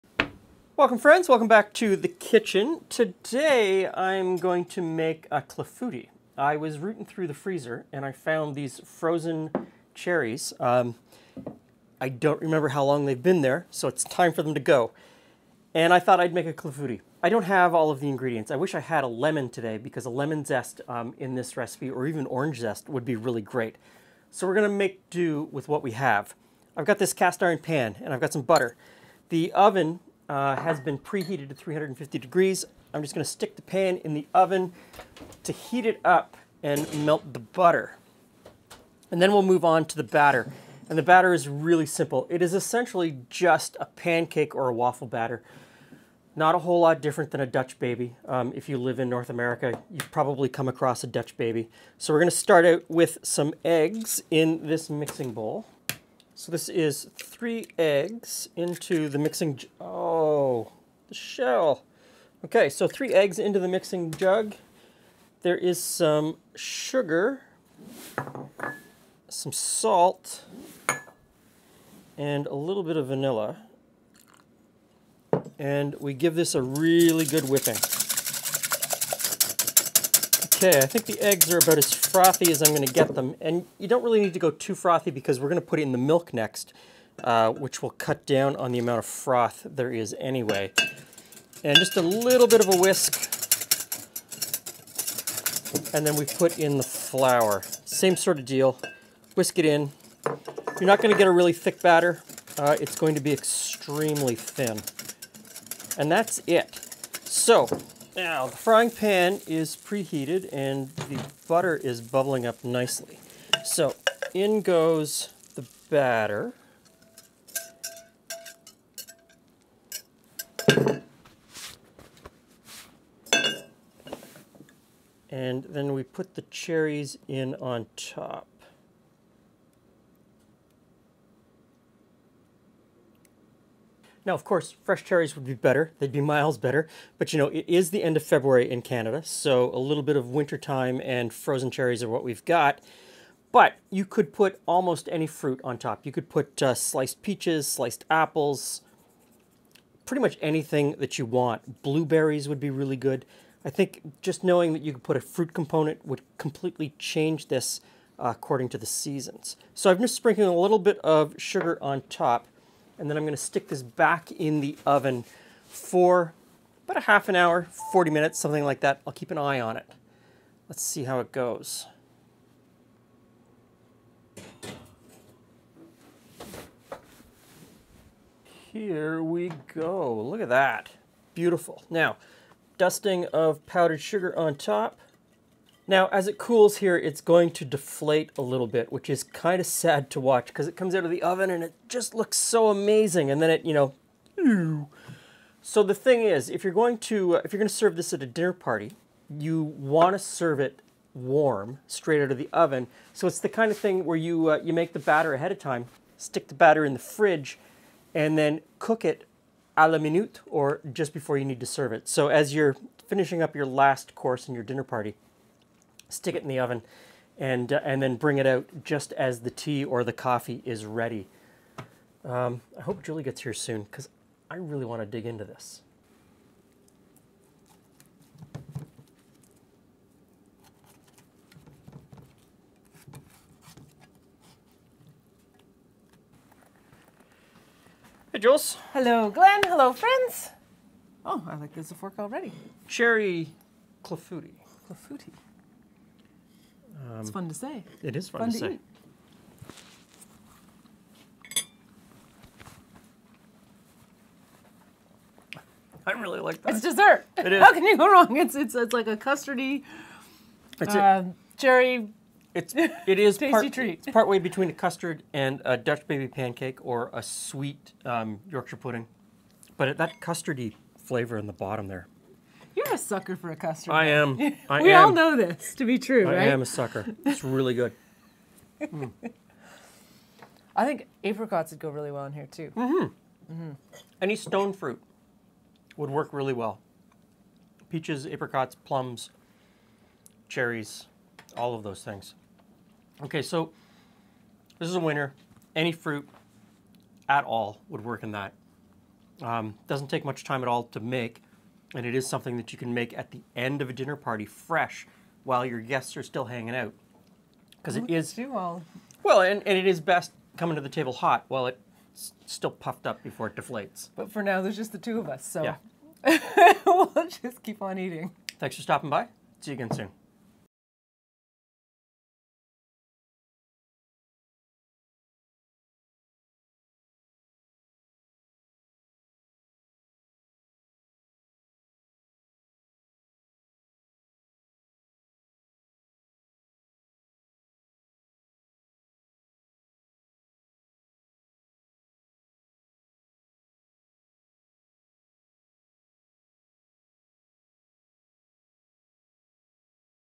Cherry Clafoutis Recipe - Glen And Friends Cooking - How To Make Cherry Clafoutis aux Cerises
Cherry Clafoutis Recipe - Glen And Friends Cooking
What is a Clafoutis or a Clafouti? Where does the Clafoutis recipe originate and what is in a clafoutis? The clafoutis recipe is a traditional French dessert recipe that seems to originate from the Limousin region. A clafoutis is essentially a pancake or waffle batter poured into a hot buttered pan with cherries strewn on top and then baked. Cheries are so linked to the dish, it is most often called a cherry clafoutis recipe. Most would argue that the cherries should be whole and include the pits - and that no other type of fruit should be used in the dessert recipe... If another fruit is used then it would instead be called a Flognarde.
The origin of the word Clafoutis is argued about as well - According to some, the word originally came from the Occitan word clafir or claufir; meaning “garnish” or “filling”. Others, would argue that the origin stems from the Latin expression clavum fingere, meaning “to plant a nail”. Apparently referring to the cherries, which are “planted” in the dough.
There are many variations on the clafoutis dough, or clafoutis batter, but most are thin. There are also differing thoughts on how to bake a clafoutis - but any way you make it or slice it... clafoutis is amazing.

Ingredients:
30 mL (2 Tbsp) butter
3 large eggs
250 mL (1 cup) milk
5 mL (1 tsp) coarse salt
5 mL (1 tsp) pure vanilla extract
75 mL (1/3 cup) granulated sugar
125 mL (½ cup) all-purpose flour
500 mL (2 cups) pitted cherries
Icing sugar

Method:
Preheat oven to 180ºC (350°F).
Place butter in a 10" cast iron pan, and stick it in the oven.
Heat until butter has melted, and pan is hot.
Meanwhile, whisk eggs, milk, salt, vanilla, and sugar until frothy.
Add flour and whisk just until combined.
Pour batter into heated pan, and arrange the cherries on top.
Sprinkle with some granulated sugar, and bake until top is puffed and lightly golden brown, 25–35 minutes.
Serve warm with icing sugar sprinkled overtop.